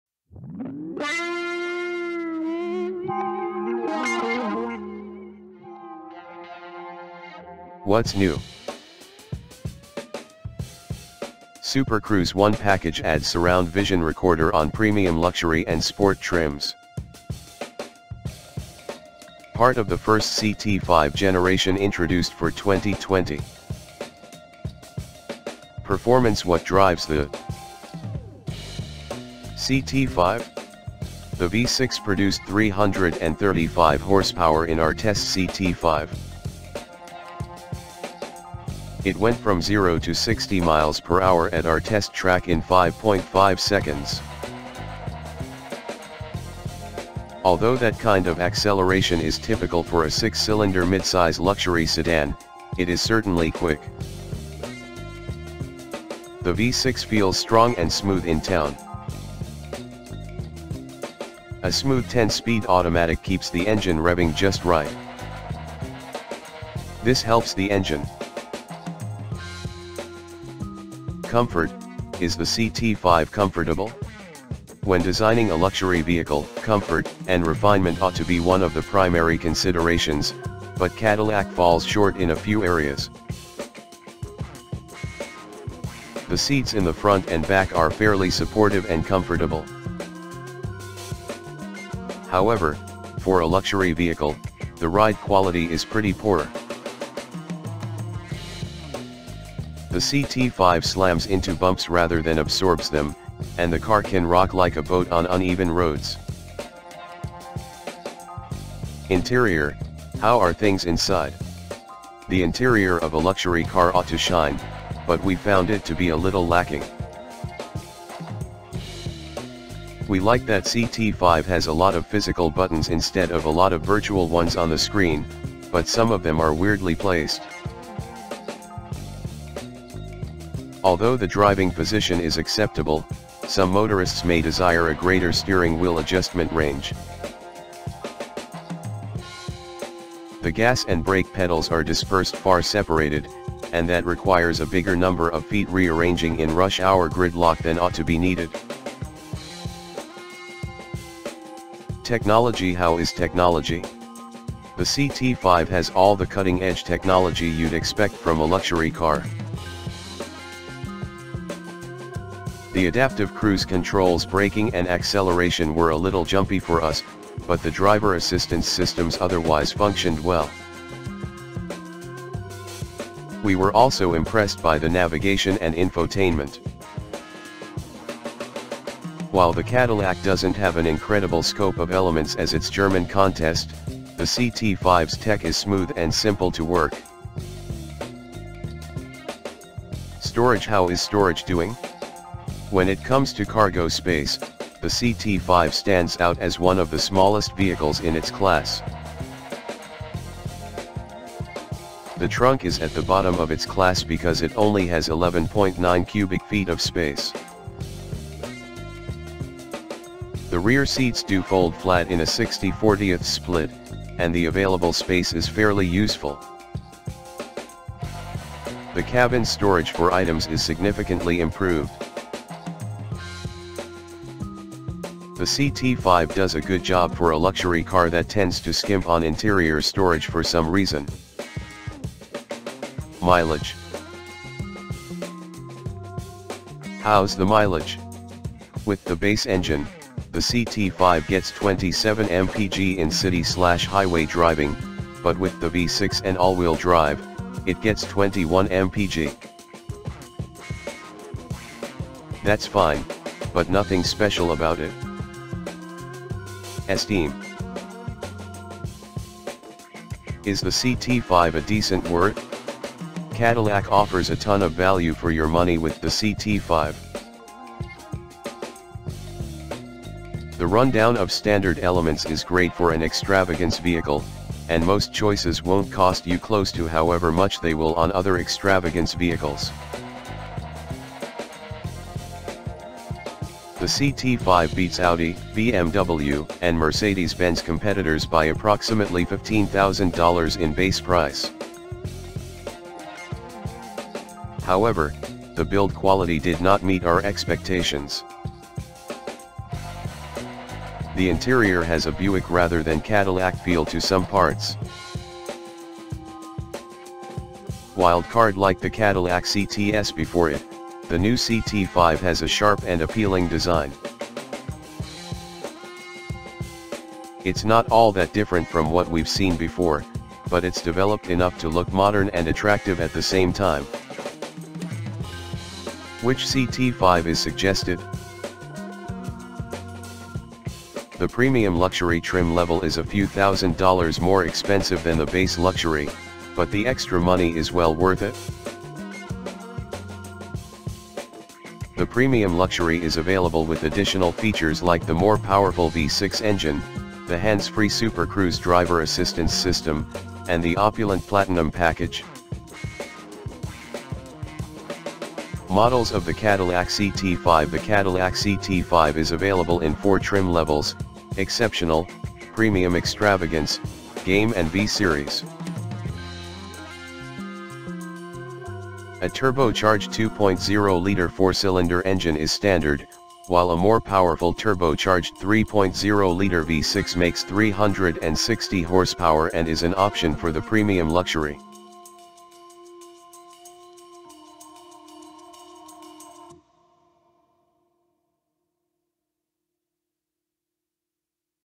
2023 Cadillac CT5 Review : A Midsize Luxury Sedan with Mixed Results
2023 Cadillac CT5 Review
How does the CT5 drive? Our test CT5 had the 335-horsepower V6.
At our test track it accelerated from 0 to 60 mph in 5.5 seconds.
It's certainly quick, though that kind of acceleration is typical for a midsize luxury sedan with six-cylinder power.
Around town, the V6 feels smooth and strong.
The engine is aided by a smooth 10-speed automatic that keeps the engine revving at just the right speed.
The engine's auto stop-start system is unobtrusive, so the biggest issue driving in traffic is the somewhat touchy brake pedal that can make it hard to stop smoothly.
We found the CT5's handling to be stable and secure.
The CT5 isn't a sport sedan, but it's more entertaining than it might seem thanks to a well-tuned suspension and accurate steering.